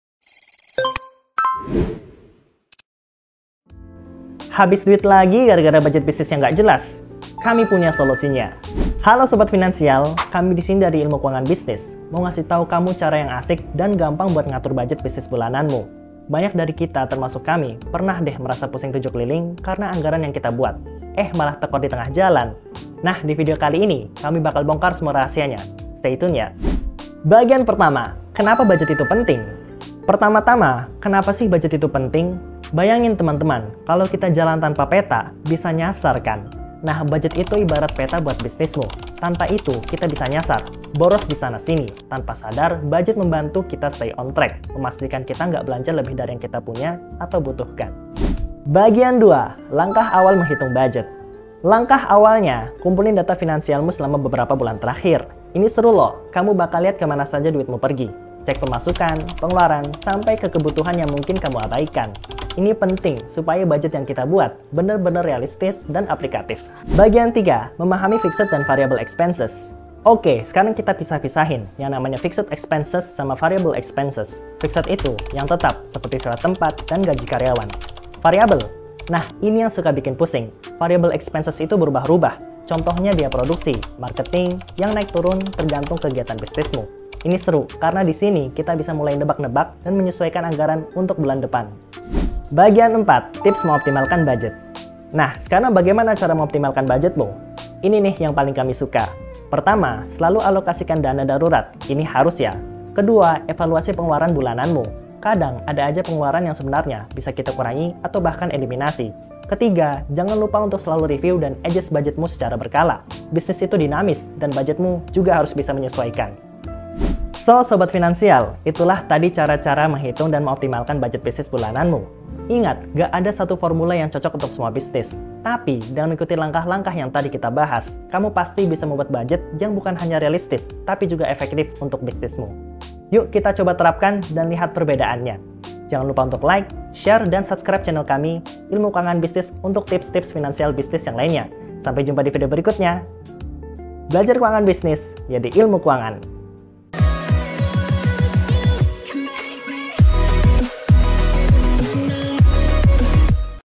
Bagaimana Cara Menghitung Budget Bisnis Bulanan dengan Tepat? - Ilmu Keuangan
Join Telegram dan Dapatkan:
✅E-Book Gratis
✅Finance Tools Gratis
✅Weekly Article

Sekarang kamu bisa mengelola inventory, keuangan usahamu salam satu aplikasi. Hmm gimana caranya?